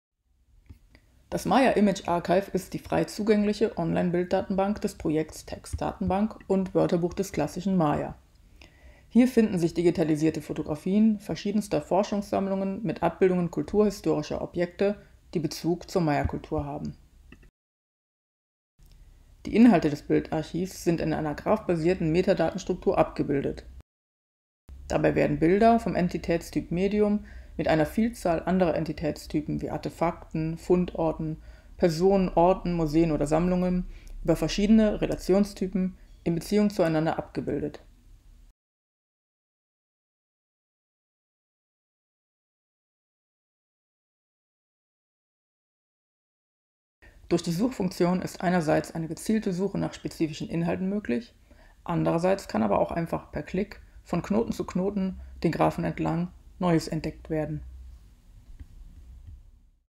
Coding da Vinci Niedersachsen 2020 – Niedersächsische Staats- und Universitätsbibliothek Göttingen B
Coding da Vinci Niedersachsen 2020 – Präsentation des Datensets "The Maya Image Archive" der Niedersächsischen Staats- und Universitätsbibliothek Göttingen